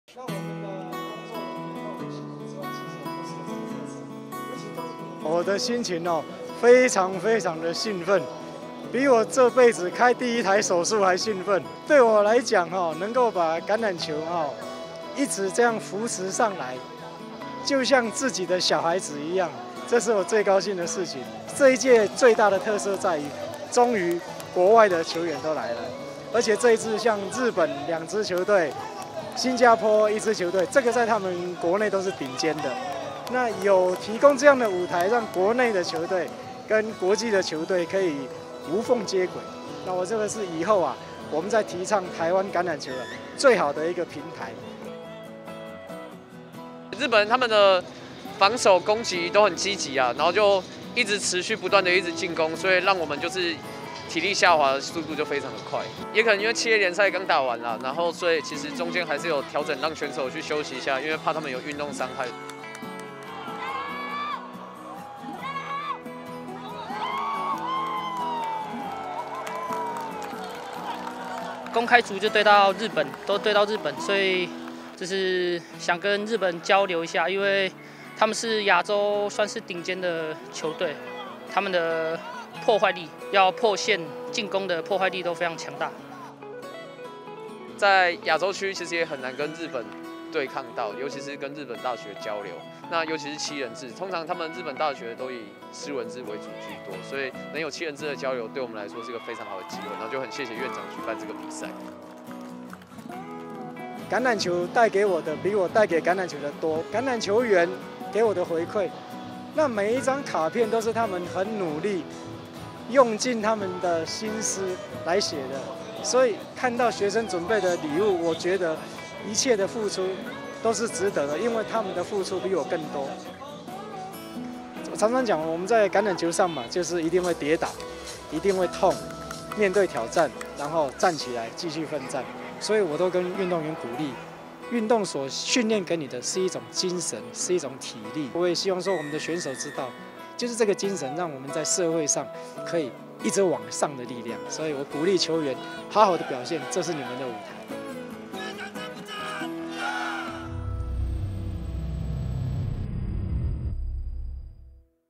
《橄欖球》2023元坤盃首邀國外球隊 接軌國際助球員成長
2023年元坤盃大專橄欖球邀請賽12月23、24日在台北田徑場開打，元坤盃已邁入第四年，本屆首次邀請來自國外球隊共同切磋球技，公開組有日本關西大學、日本拓殖大學，以及一般組新加坡南洋理工大學，共3國15所學校參賽，元坤盃創辦人暨義大醫院院長杜元坤期許為國內球員提供舞台，無縫接軌國際。

預賽與兩支日本球隊同在C組的臺北市立大學一日兩戰，上午首戰以15：5輕取文化大學，下午對上關西大學，上半場靠著石家榮與張正岡兩度達陣與加踢攻下12分取得領先，可惜下半場遭關西大學逆轉，以12：17吞敗。

看著球員在場上奔馳，元坤盃創辦人暨義大醫院院長杜元坤直言：比開第一台手術還興奮！他在開幕典禮時親自表演小提琴，並捐贈28所國高中學校各10萬元發展基金，其中有貼心的學校回送感謝信，也讓他心裡感覺暖暖的，他說：「我常說橄欖球帶給我的比我帶給橄欖球的多，球員給我的回饋，每一張卡片都是他們用心來寫，看到學生準備的禮物，我覺得一切的付出都是值得的。」